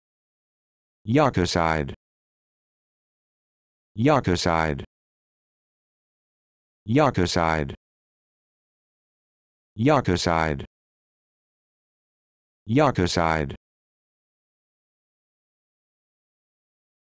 How to Pronounce Yuccoside
This video shows you how to pronounce Yuccoside